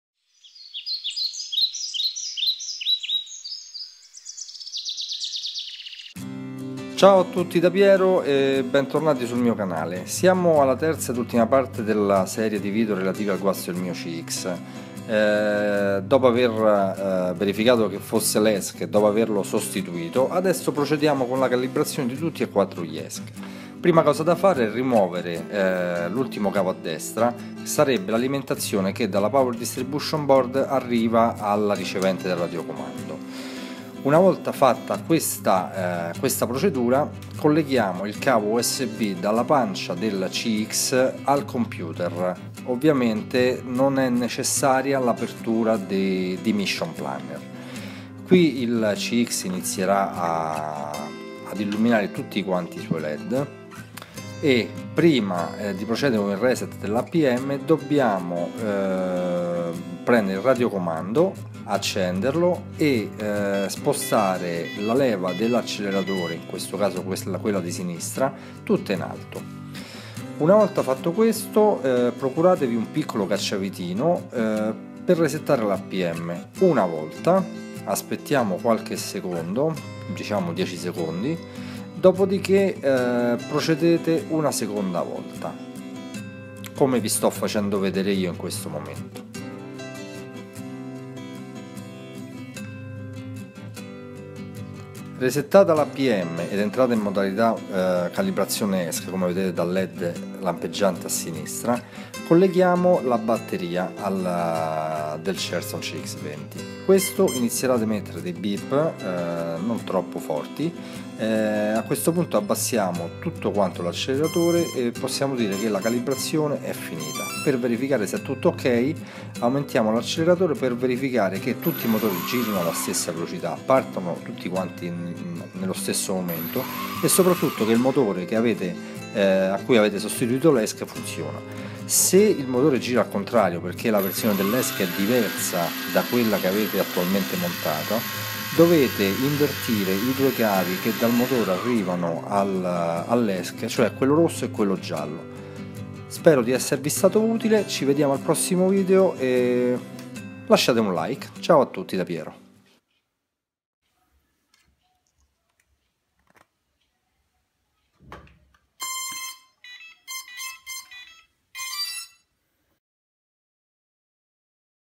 Come Calibrare gli ESC del Cheerson CX-20
Dopo avervi mostrato nella prima e nella seconda parte come verificare se il guasto è l'ESC o il MOTORE, in questo video vi mostro come calibrare gli ESC dopo la sostituzione di quello guasto.